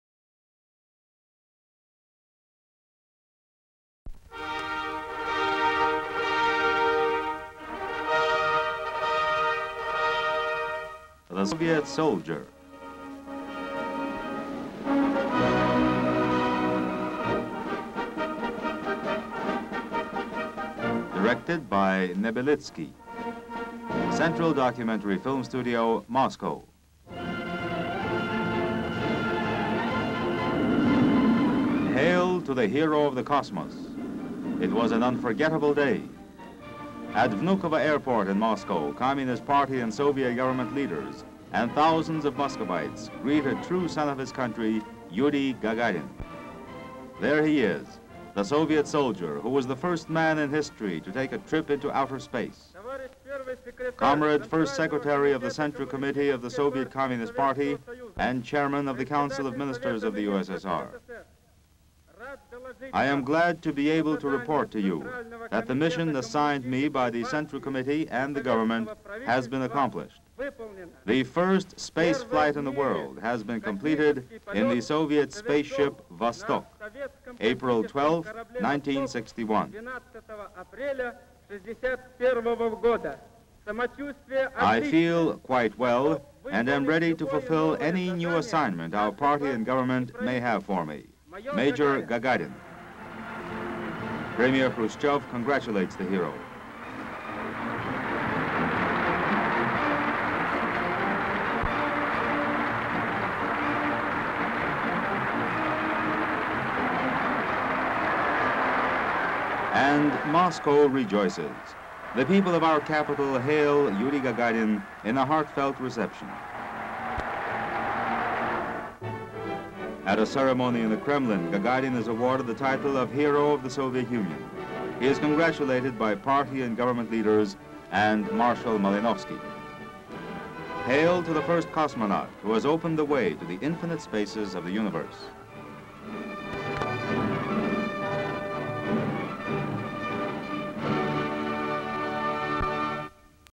Soviet Union crowd celebrates cosmonaut Yuri Gagarin in Moscow following his succ...HD Stock Footage
Soviet Union crowd celebrates cosmonaut Yuri Gagarin in Moscow following his successful first trip by a human into outer space

Welcome of Soviet cosmonaut Yuri Gagarin in Moscow, after he became the first human to voyage into outer space, orbiting the earth in his Vostok spacecraft. From a Russian newsreel with Enlish narration. Yuri Gagarin arrives by a plane at a Moscow airfield. Premier of the Soviet Union, Nikita Khrushchev, greets Gagarin. A huge crowd holds pictures and banners of Gagarin and Khrushchev. Gagarin reports to Premier Khrushchev that he has fulfilled his mission and feels well and ready for his next assignment. Gagarin greets his wife. He moves in a car through crowd filled streets. At a ceremony in the Kremlin Premier Khrushchev and Defense Minister Marshal of the Soviet Union Rodion Malinovsky congratulate Gagarin and award him the title "Hero of the Soviet Union". Location: Moscow Russia Soviet Union. Date: April 1961.

57,000+ broadcast-quality historic clips for immediate download.